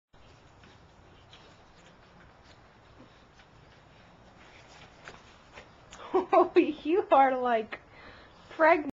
Bobby after eating cat food
Bobby ate an entire dish of cat food.  And his belly showed it.